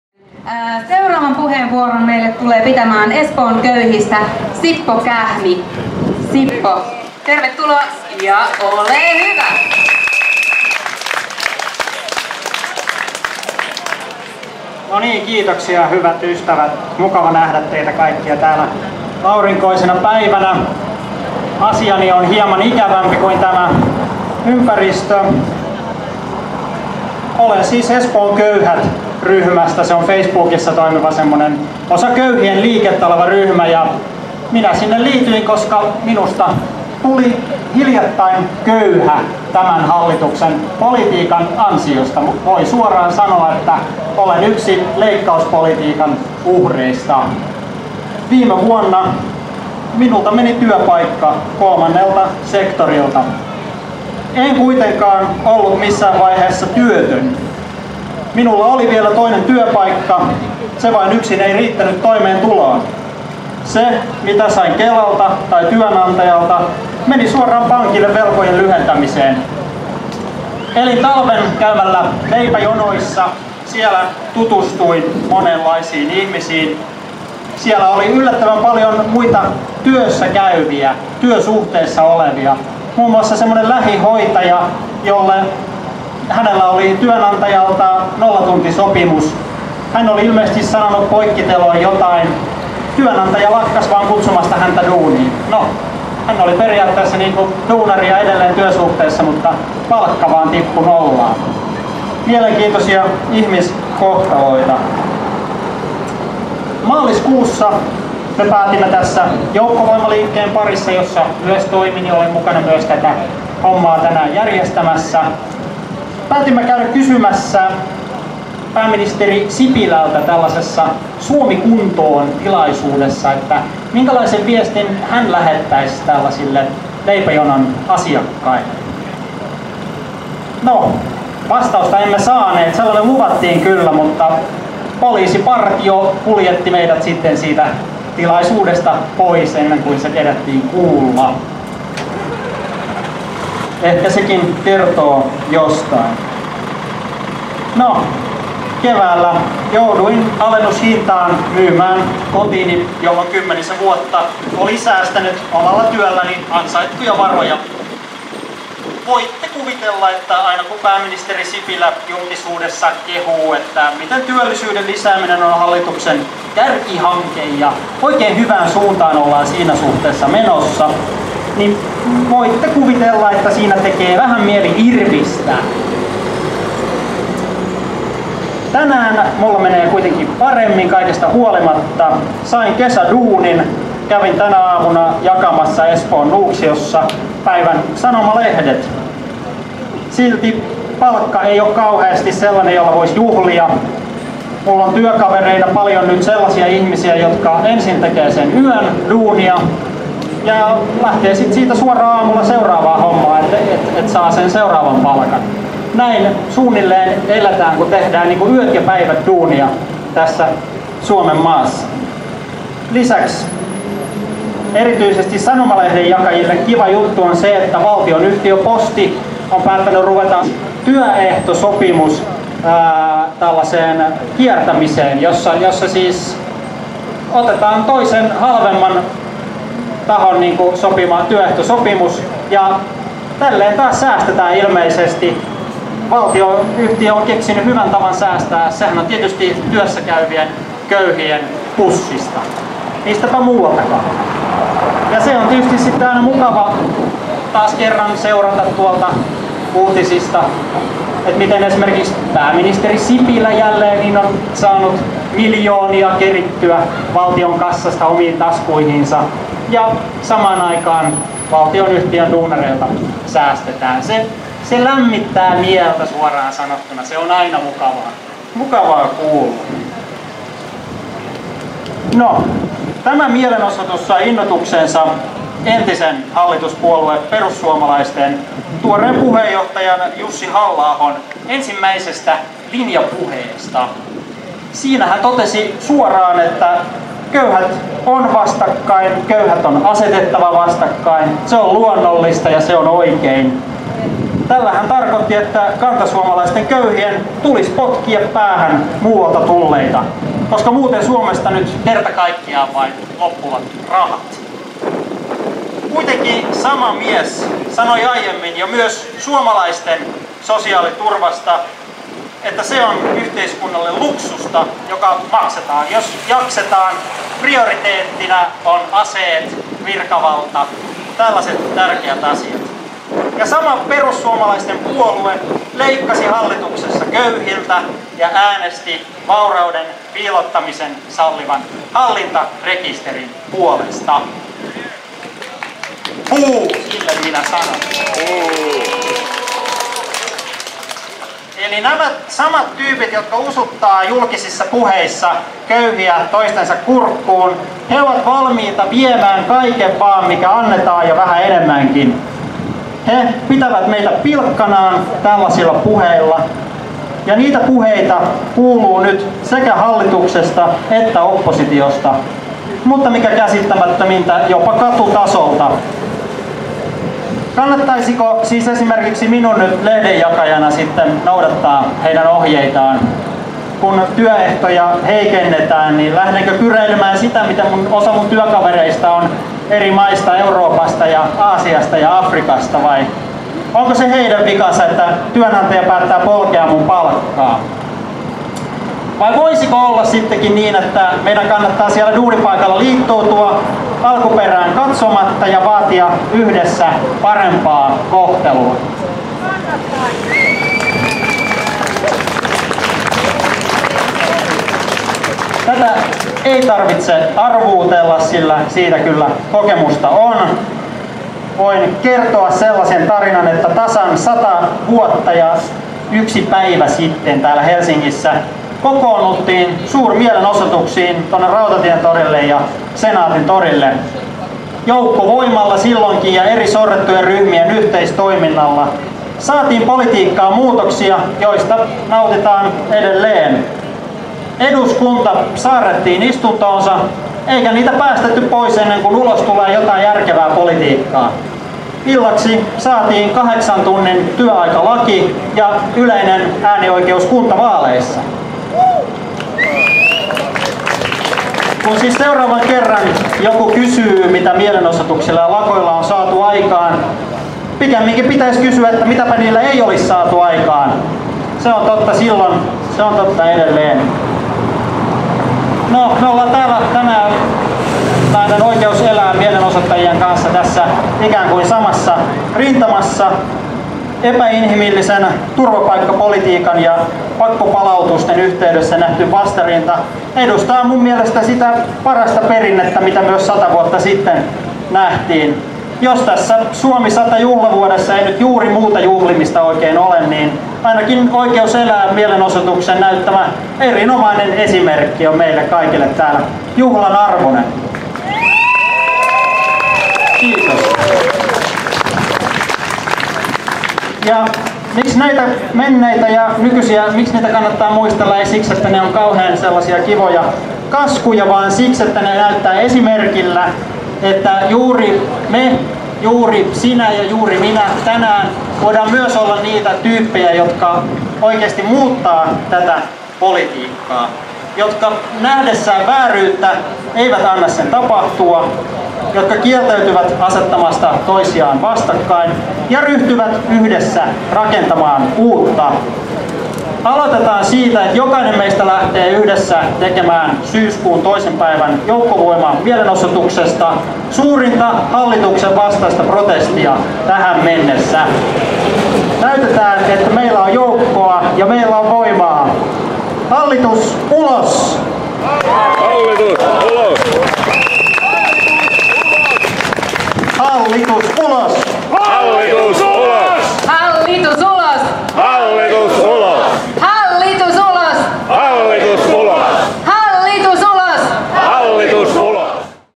Ei köyhiä vastakkain! (Sippo Kähmi)
Sanomalehdenjakaja Sippo Kähmi Espoon köyhät-ryhmästä piti puheenvuoron Pääkaupunkiseudun Joukkovoiman mielenosoituksessa Helsingissä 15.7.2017.